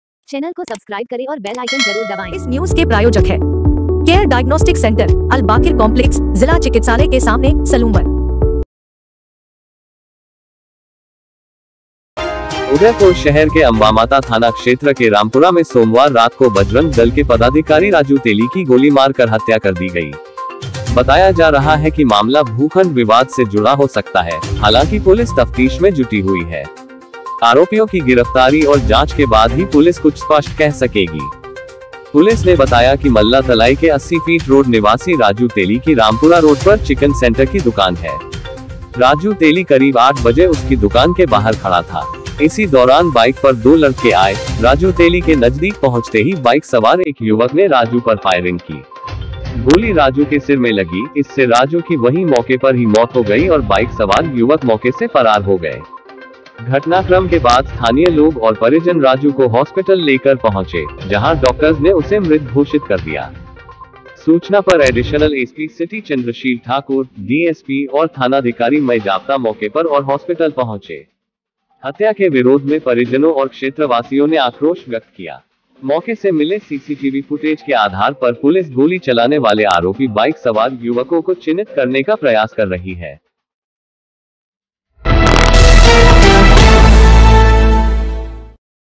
udaipur breaking news !! बजरंग दल के राजू परमार तेली की गोली मार कर हत्या
udaipur me raju teli ki hatya